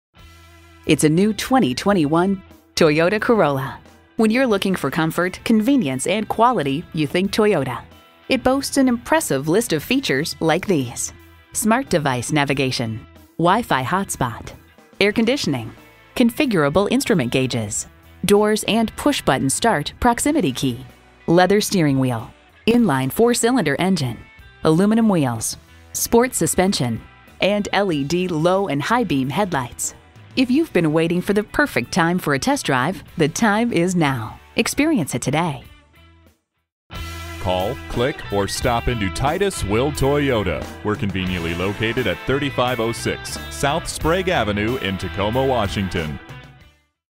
2021 Toyota Corolla Tacoma, WA
Year: 2021
Make: Toyota
Model: Corolla
Trim: SE
Engine: 4 2.0 L
Transmission: Variable
Color: Blue
Interior: Blk Premium Fabric
Stock #: T210722
VIN: JTDP4MCE3MJ069007

Address:
3506 S. Sprague Ave